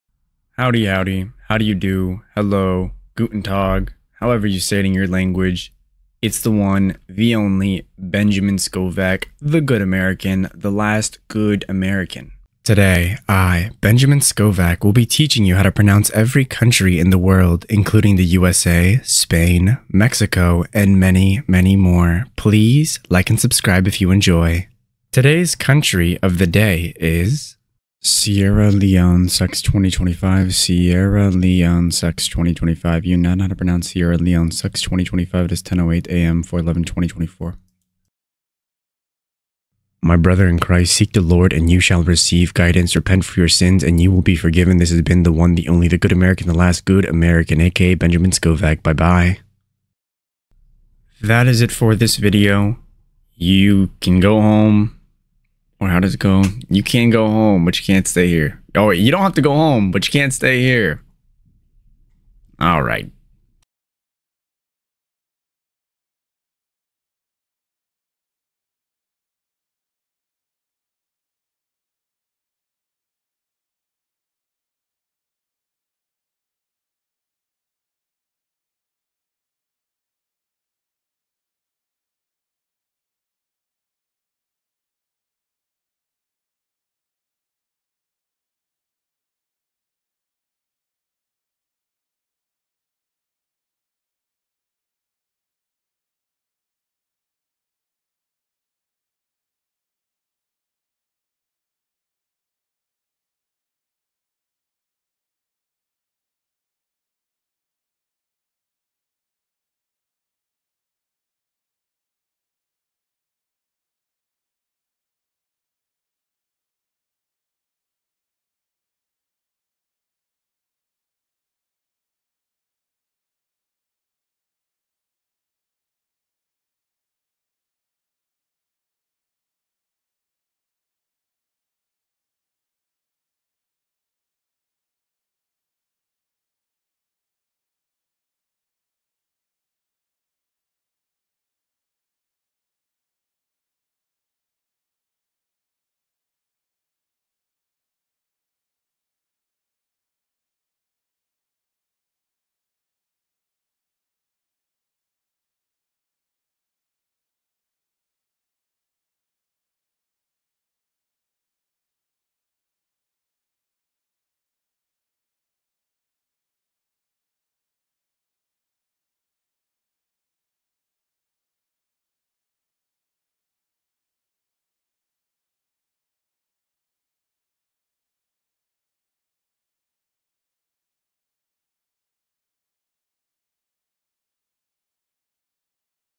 How to pronounce Sierra Leone SEX 2025?(CORRRECTLY)
Welcome!
Throughout the entirety of my channel, you will learn to pronounce vocabulary the correct way. In today’s video, you will learn to pronounce Sierra Leone SEX 2025 the correct way.
Timestamps below:
0:00 intro
0:27 word of the day
0:37 examples of word
0:51 ending

thelastgoodamerican
lastgoodamerican
benjaminscovach